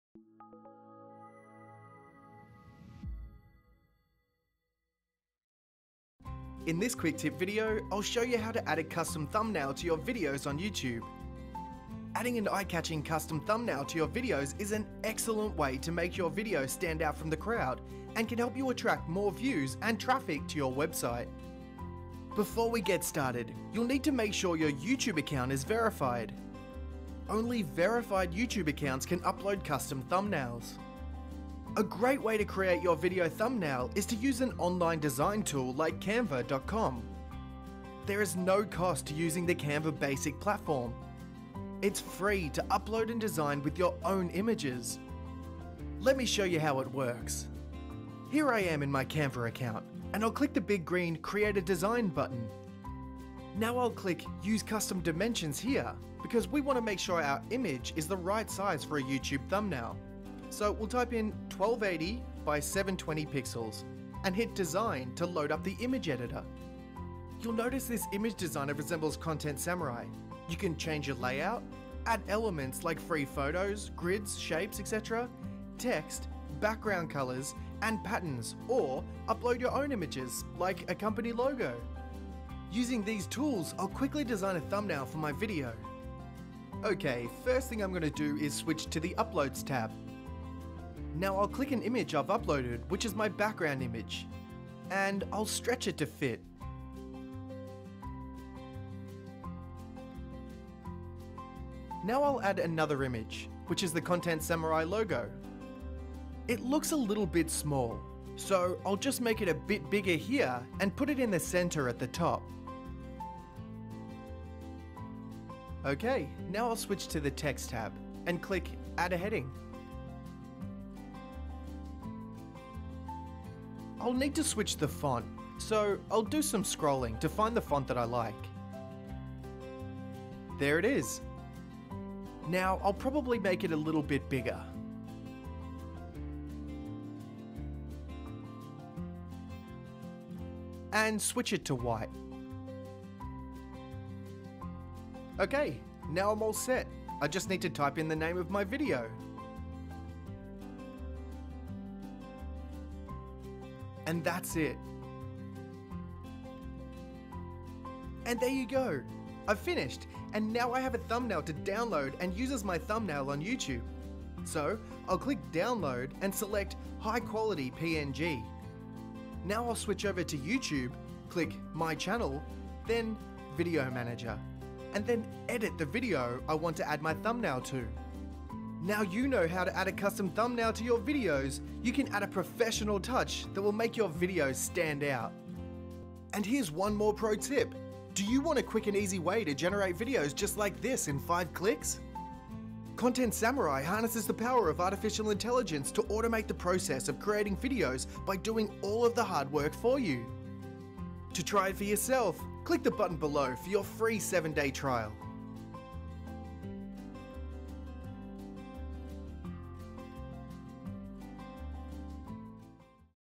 A Simple Trick To Make Your Video Stand Out From The Crowd
In this quick tip video we show you how to add a custom thumbnail to your videos on YouTube.